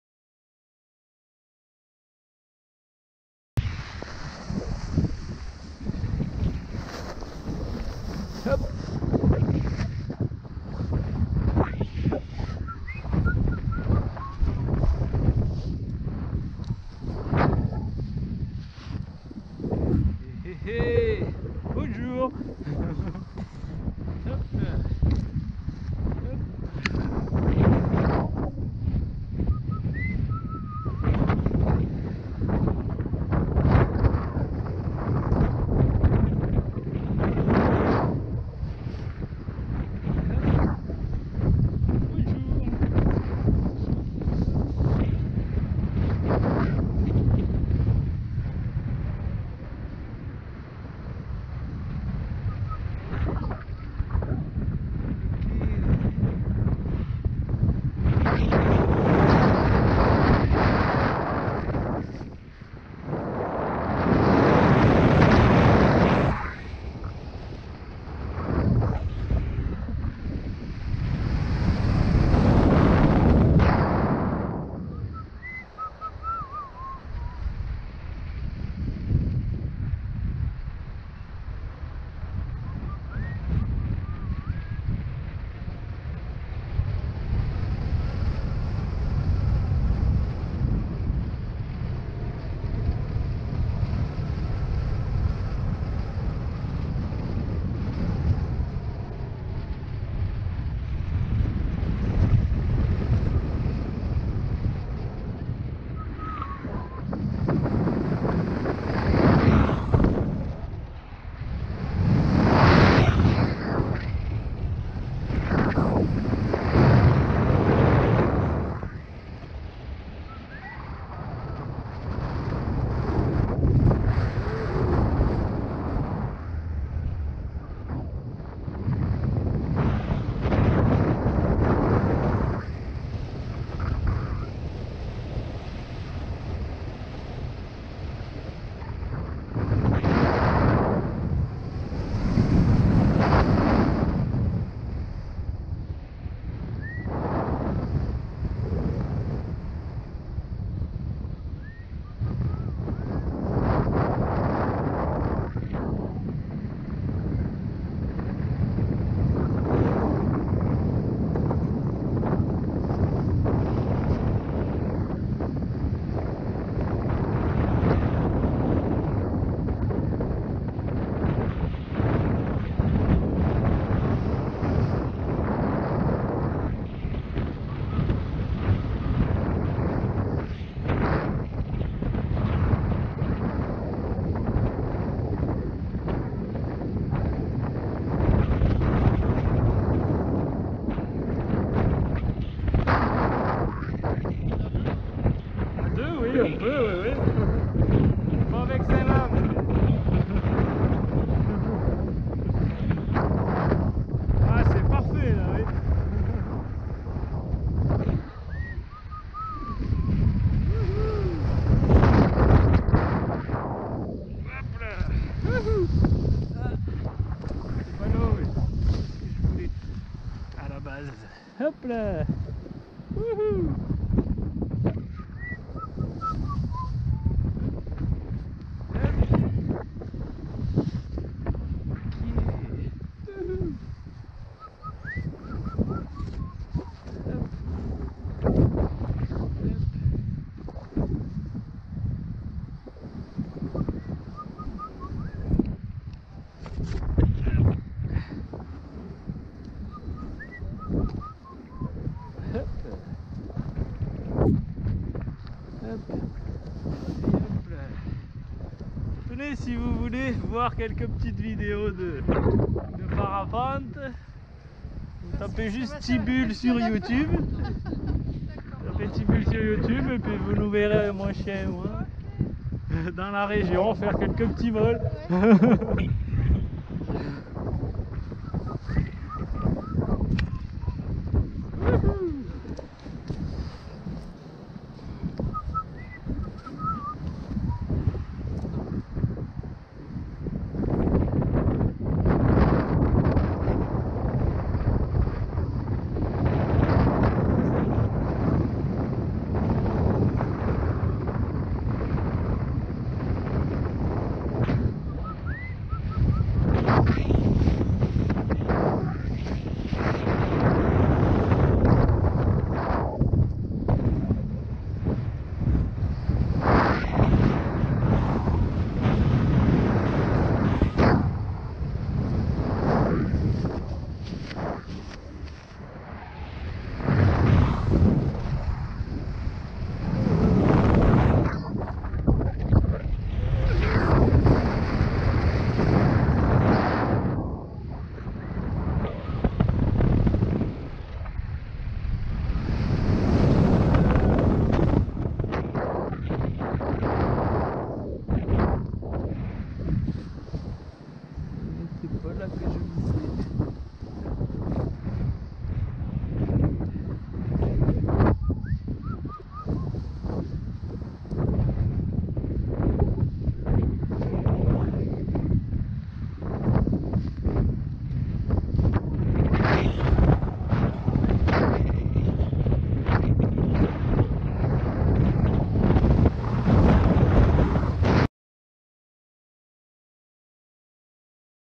Parapente Soaring pas extreme à la cool Supair EIKO20 Roc de Granville 2/5 - Normandie France
Déco : N
C'est GRATUIT et ça fait PLAISIR!!!
Même un p'tit LIKE ;-) Cool Up